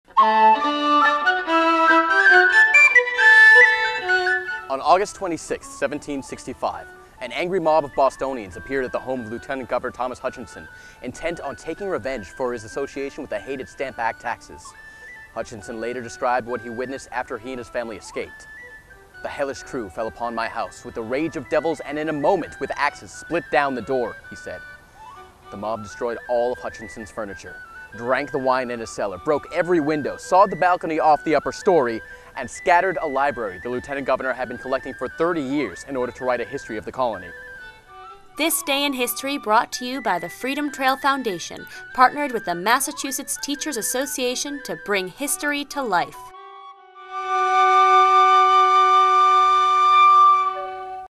This Day In History - August 26 - Freedom Trail Foundation
Freedom Trail Player Tim Hoover , who portrays Josiah Quincy, recounts the destruction of Thomas Hutchinson's house by rioters during the Stamp Act Crisis  for This Day In History!